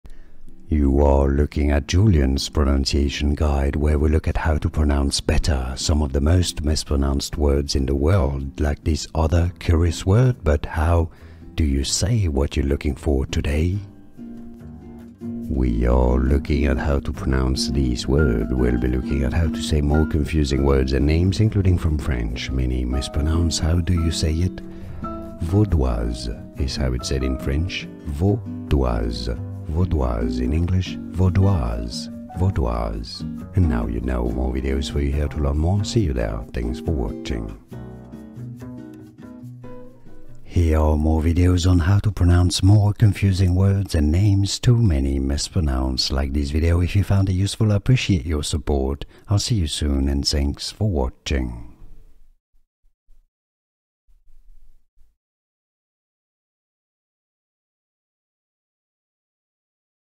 How to Pronounce Vaudoise (Correctly!)
Welcome! Here you will hear How to Pronounce Vaudoise (Correctly!) i.e. the “correct” pronunciation.

You can skip the intro through the time stamps below:
00:00 - Pronunciation Intro
00:15 - How to Pronounce Vaudoise (Correctly!)